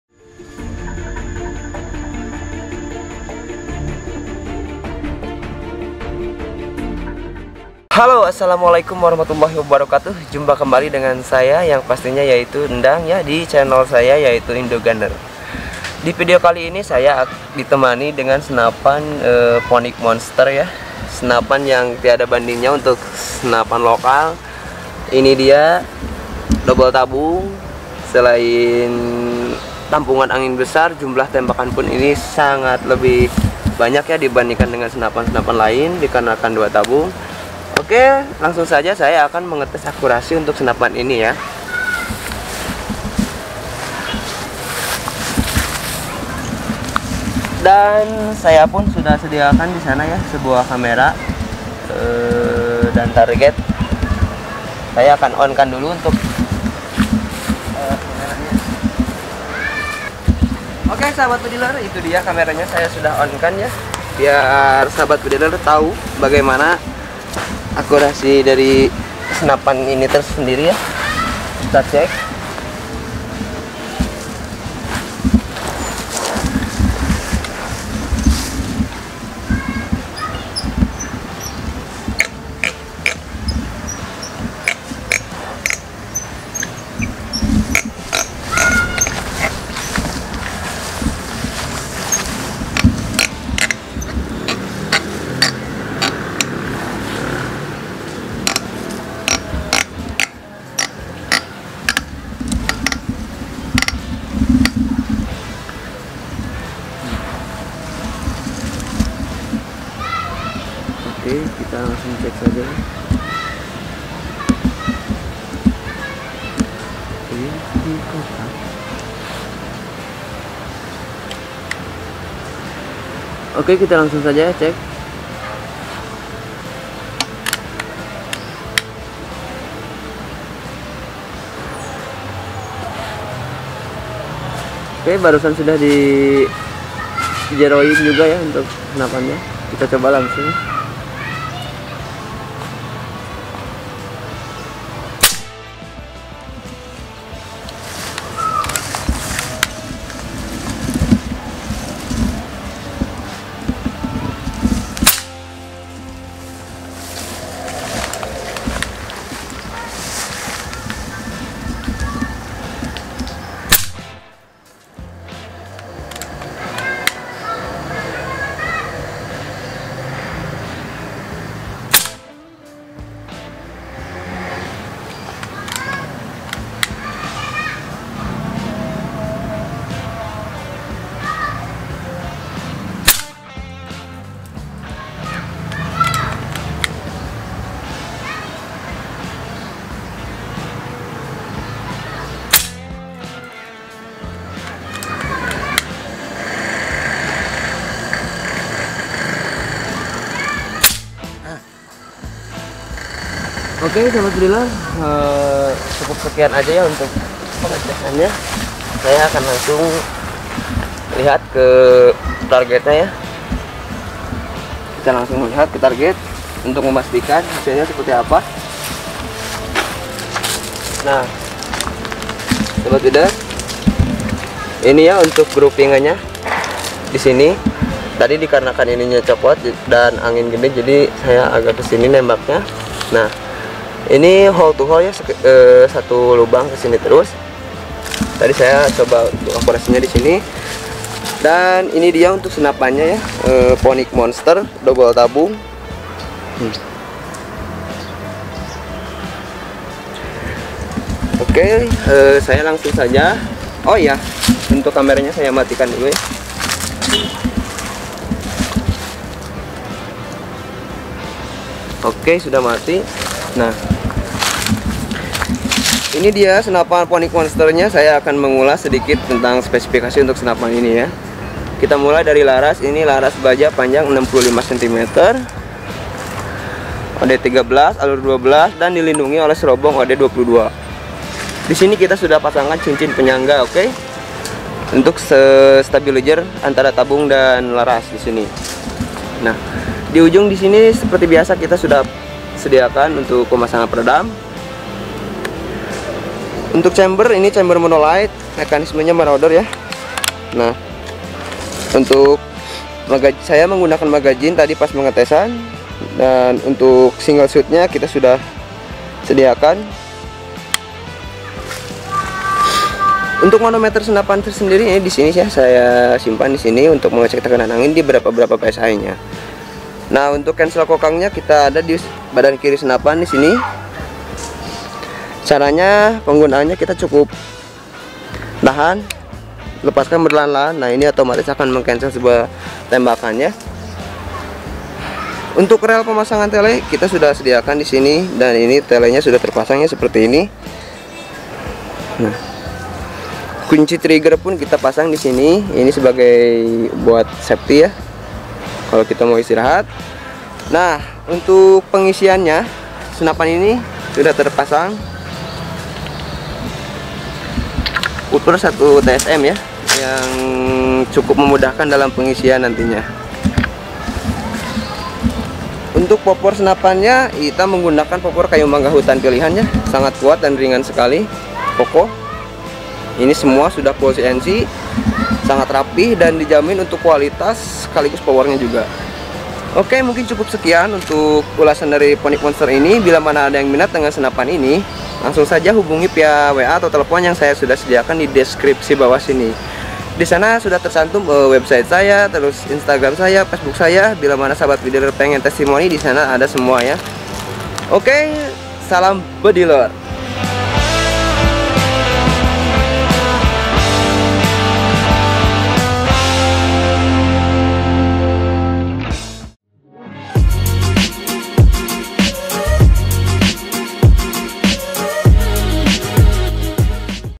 Siap Hunting Tes Akurasi Gruping Senapan Angin PCP Phoenix CNC Monsters
Terima kasih sudah menyaksikan video dari INDOGUNNERS
Tinggalkan kometar positif
Like videonya jika anda suka

Contact Person :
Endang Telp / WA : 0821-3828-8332